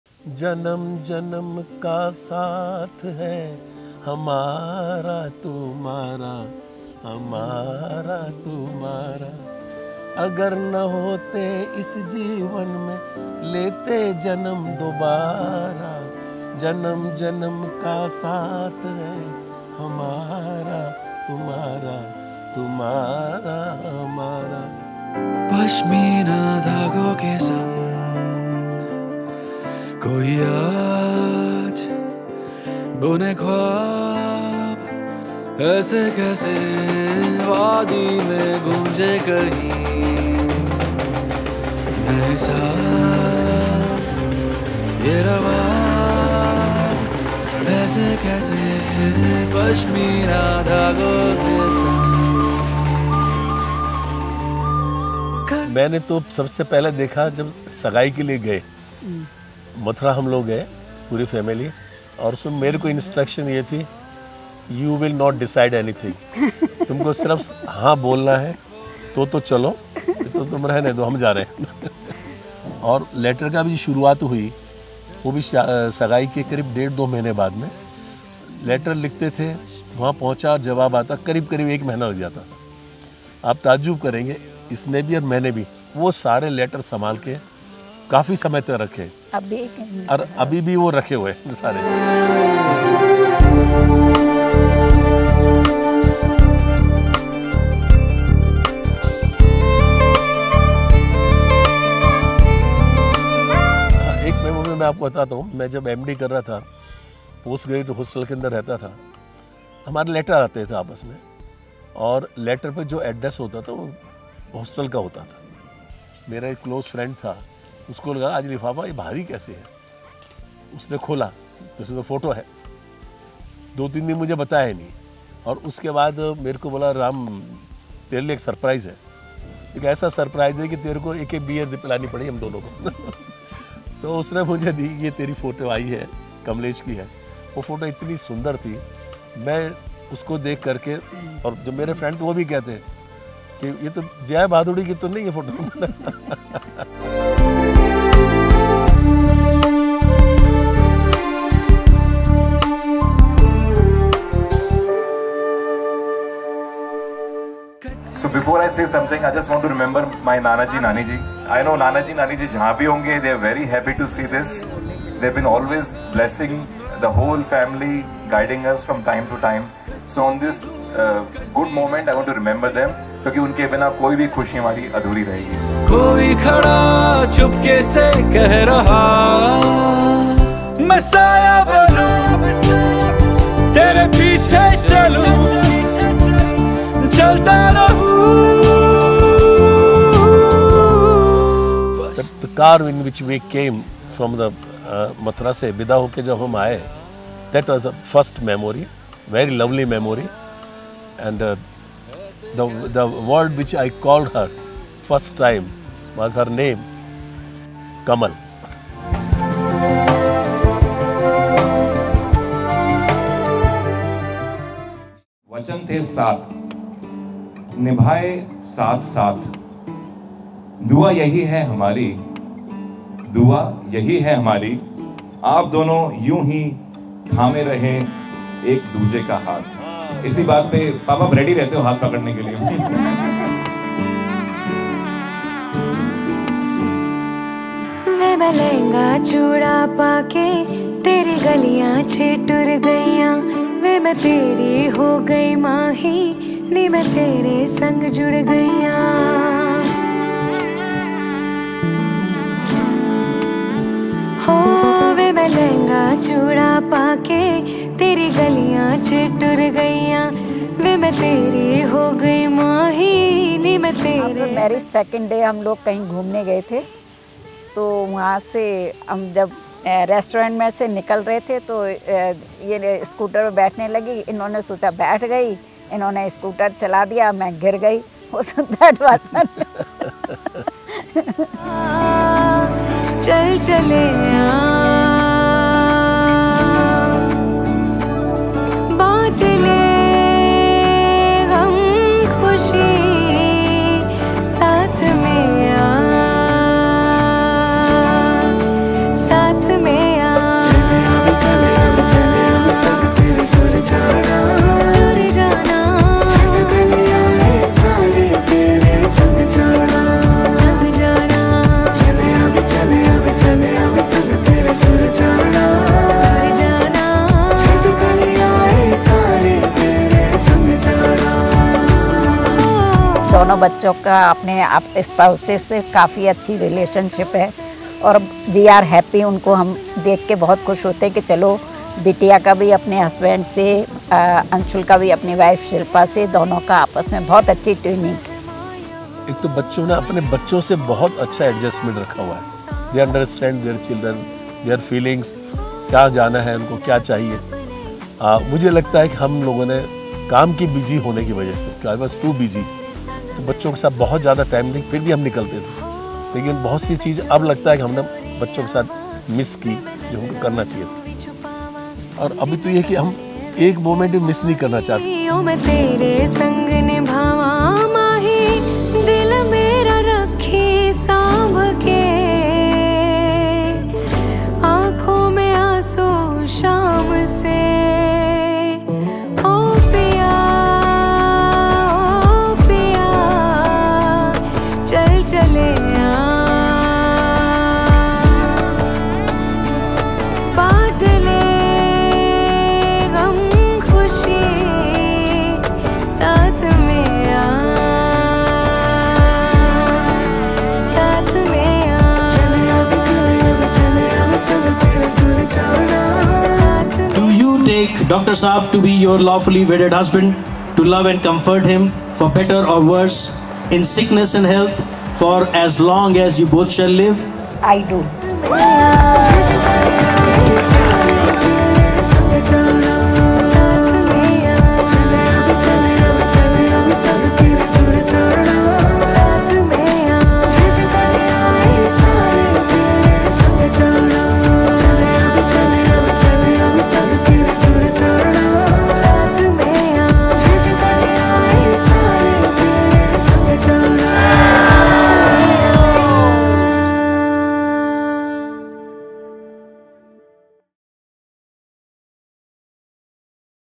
Dr. Ram & Kamlesh's Golden Journey I 50 Years of Love I Srinagar
Join us in celebrating the incredible 50-year journey of Ram and Kamlesh Ji! This heartwarming video takes you through their beautiful story, from their wedding day to raising children, enjoying vacations, and sharing future dreams together.

Watch how they celebrated their Golden Jubilee in the stunning city of Srinagar, Kashmir, at The Grand Lalit. The festivities included a joyous welcome lunch, a musical night, a serene shikara ride on Dal Lake, a touching renewal of their 7 Vows, and a memorable family gathering.

Dive into the love, laughter, and countless memories that have made their 50 years together so special. Here's to a lifetime of happiness and more to come! 🎉💖